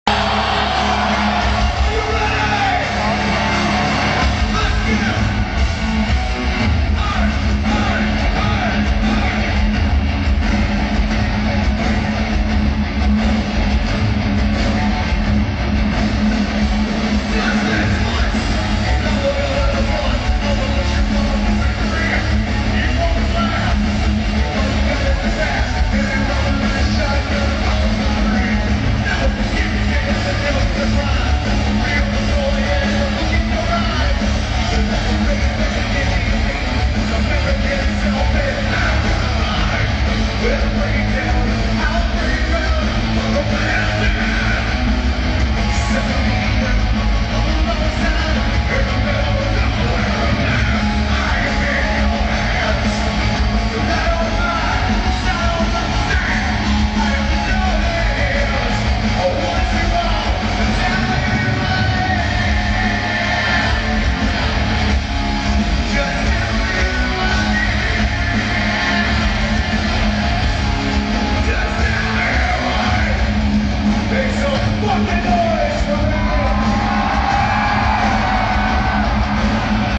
Stone Sour (Live at Uproar)
Stone Sour preforming at the Target Center (Minneapolis) at Uproar